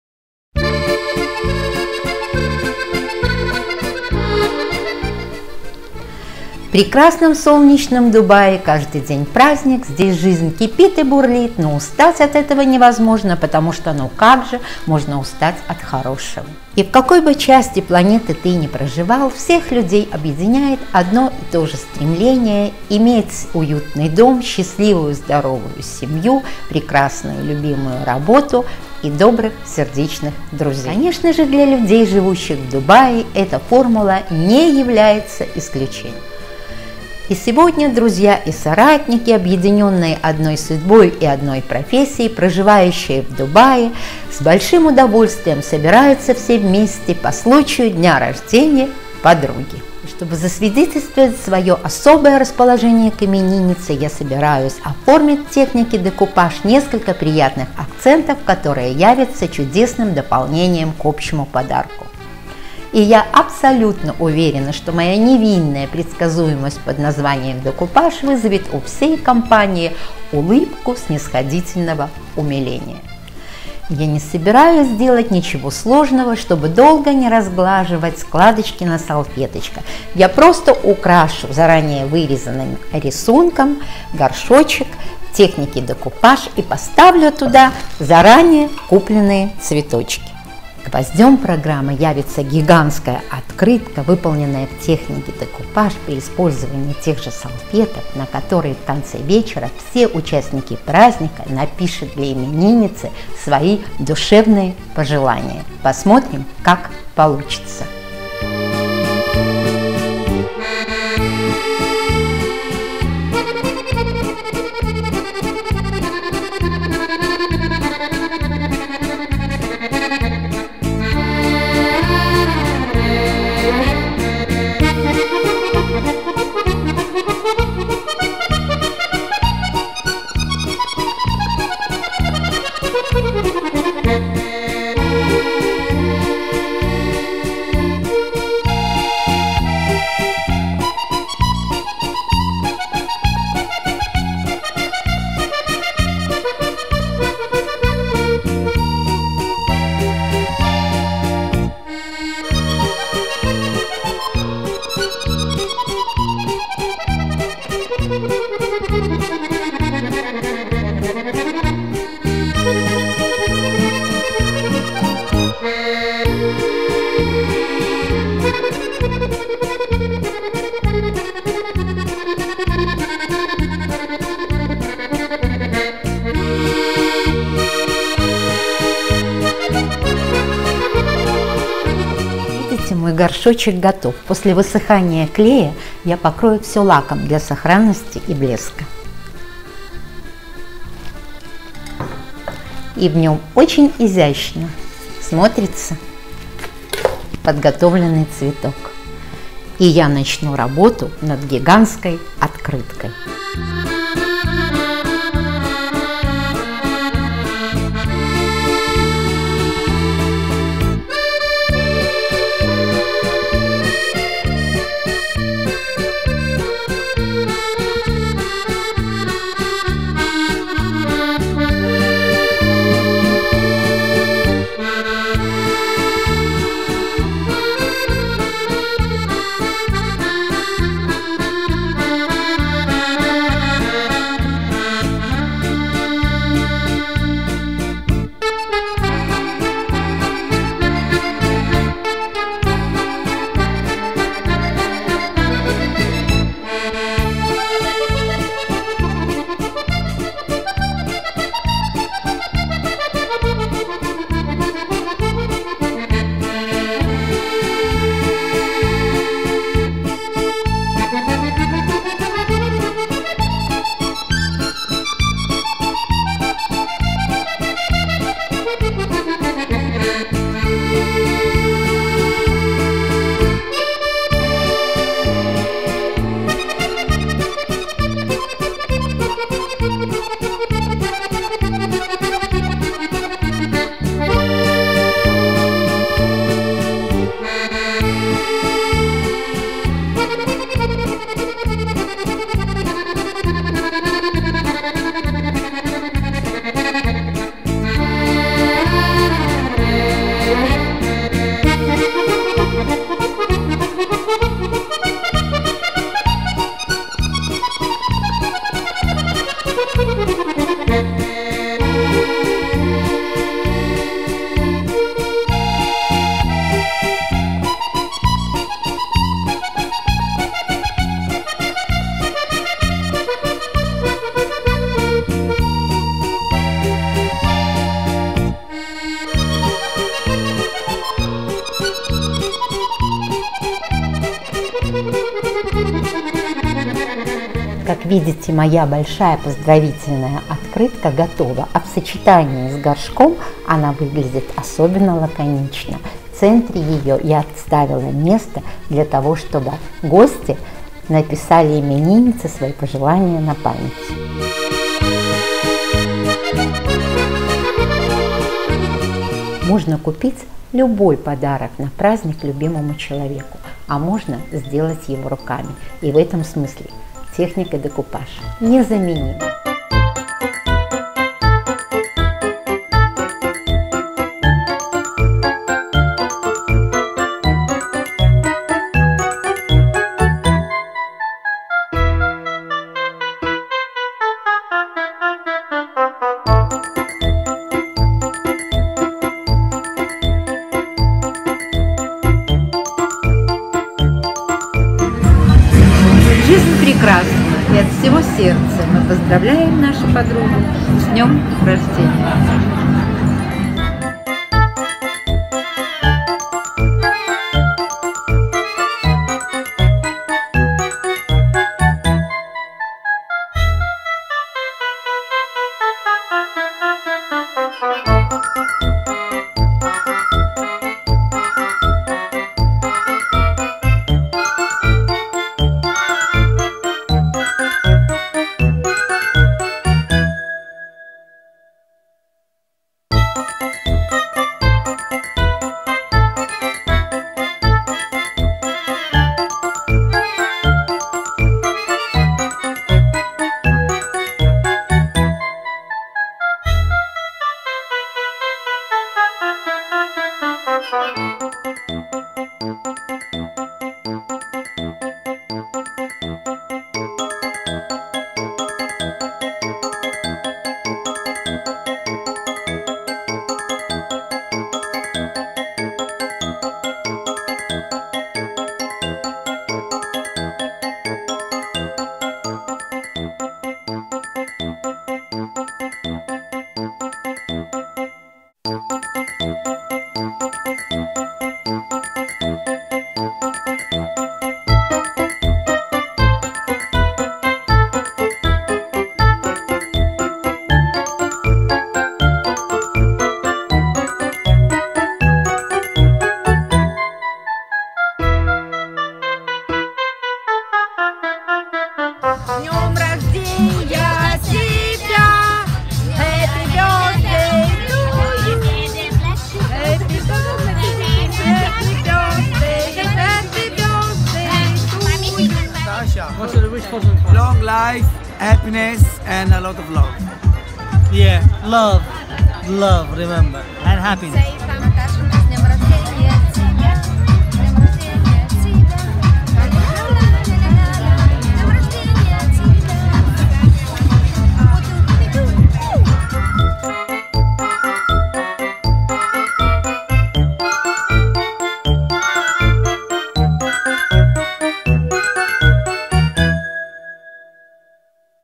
«Подарок» из Дубая выпуск 18/7
Мастер- класс по оформлению картины и цветочного горшка в технике декупаж